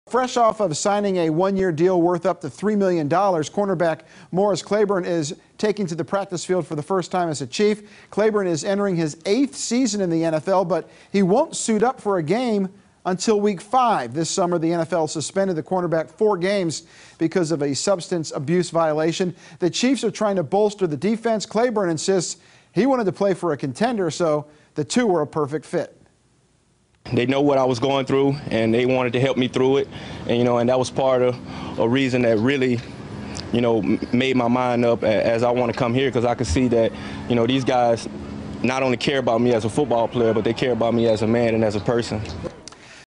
Claiborne talks about joining Chiefs
New cornerback Morris Claiborne talks about joining the Chiefs.